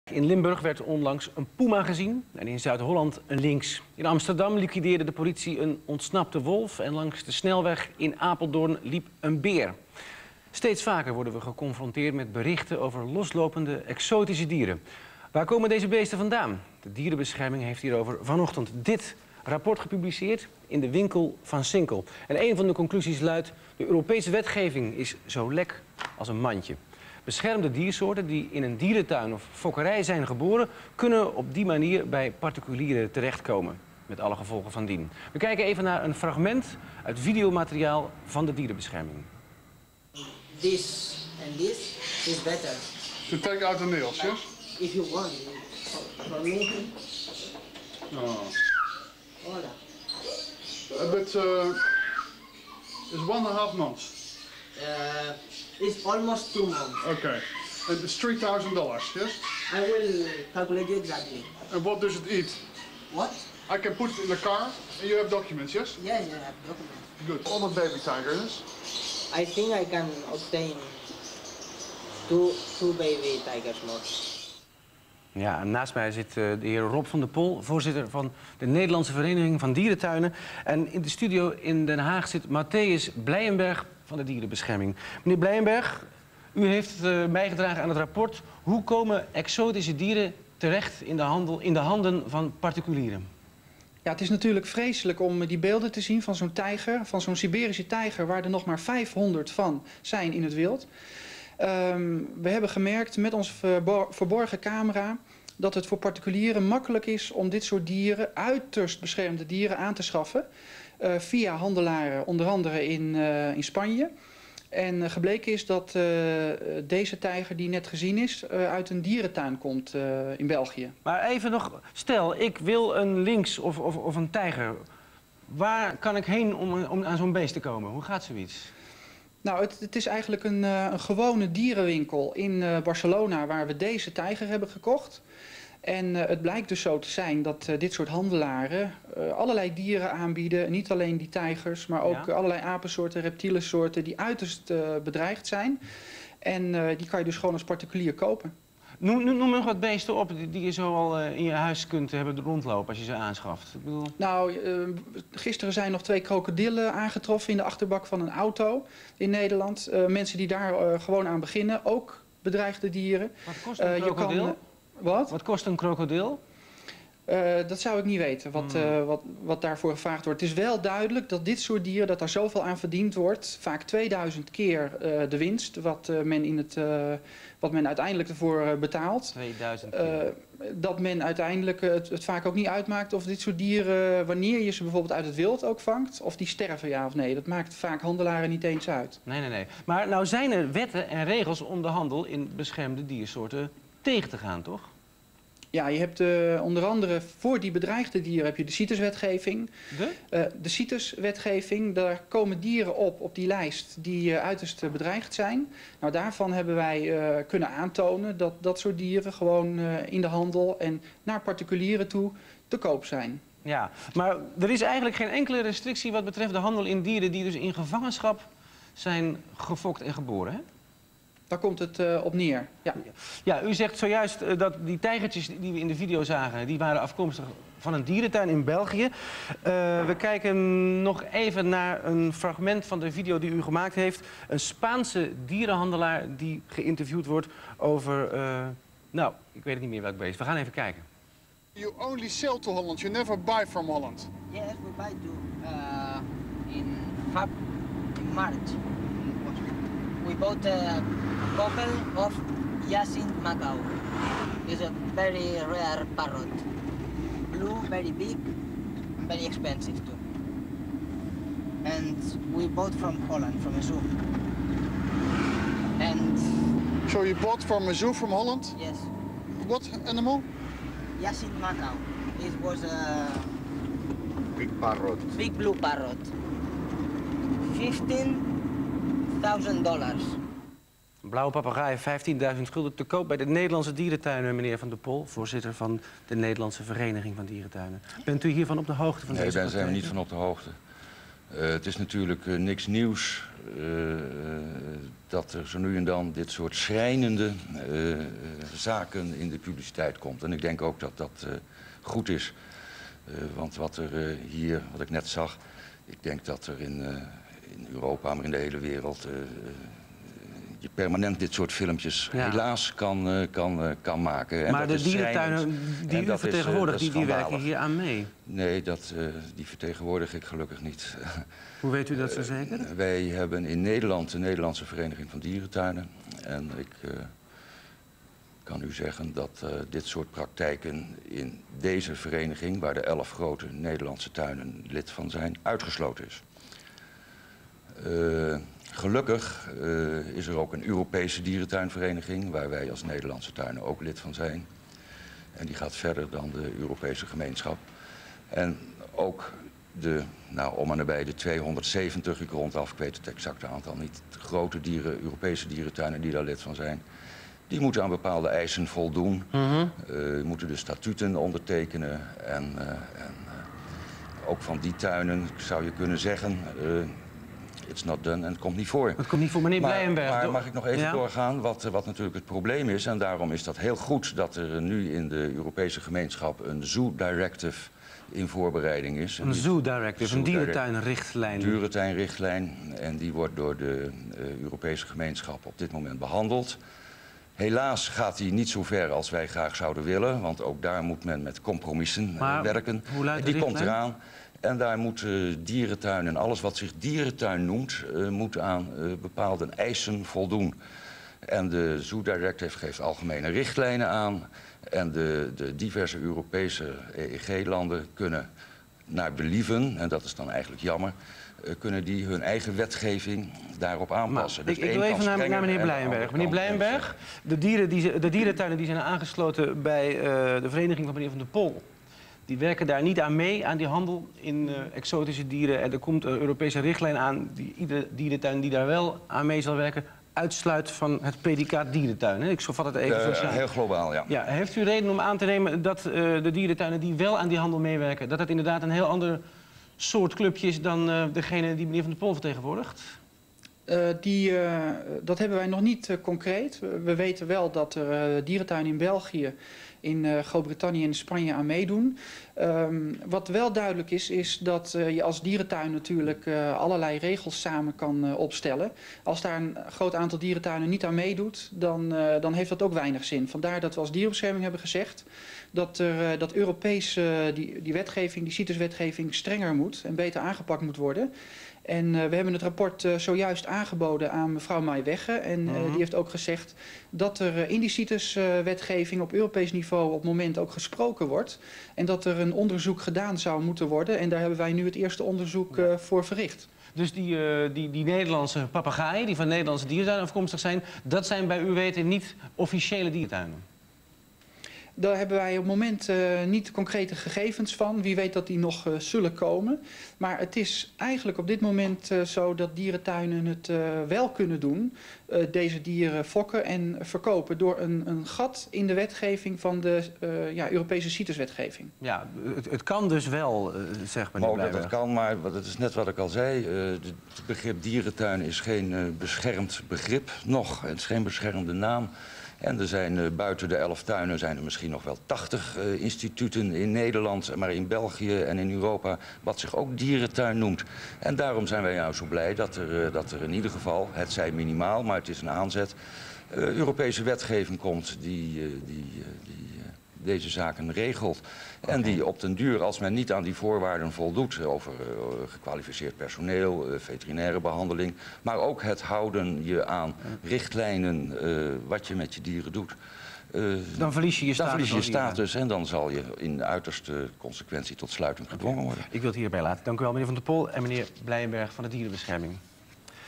Live interview Nederland 1 over de handel in exotische dieren winkel van sinkel
Live interview op Nederland 1 over de handel in exotische dieren. Door de aankoop van een siberische tijger toonden we als dierenbescherming aan dat zelfs zeer bedreigde dieren in een dierenwinkel gekocht kan worden. Dierentuinen en dierenwinkels werken mee aan de illegale internationale handel in bedreigde dieren